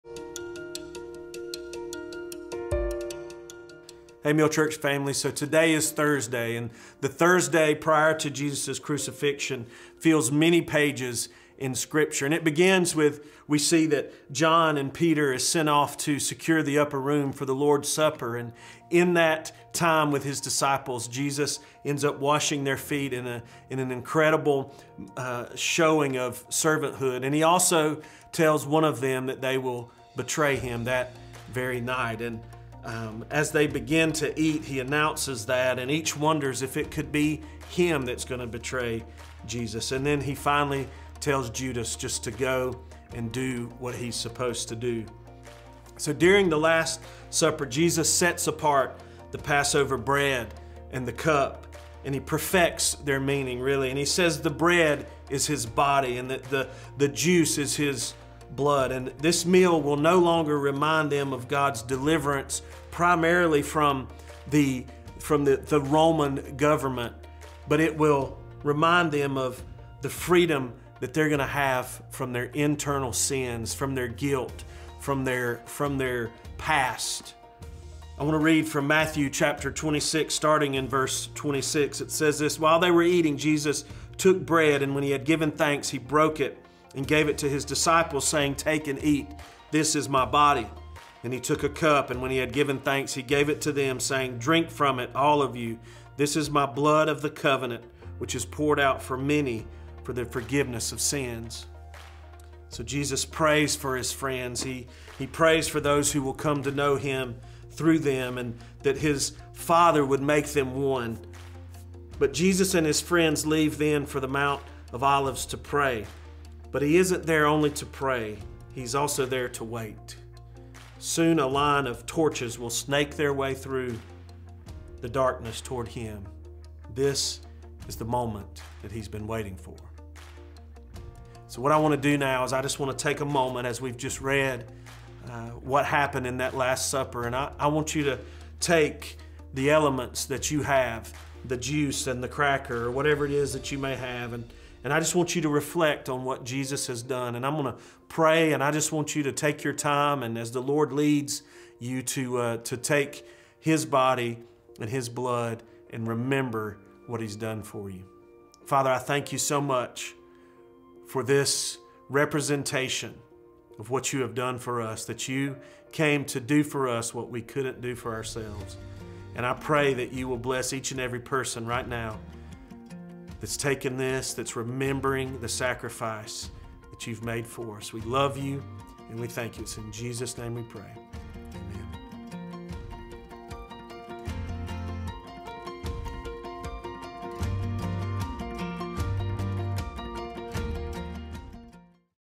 Holy Week p5
Maundy Thursday. Washing of the Feet and Last Supper of Jesus Christ with the Apostles. The word Maundy is derived from the Latin word for “command.” The “Maundy” in “Maundy Thursday” refers to the command Jesus gave to the disciples at the Last Supper, that they should love and serve one another.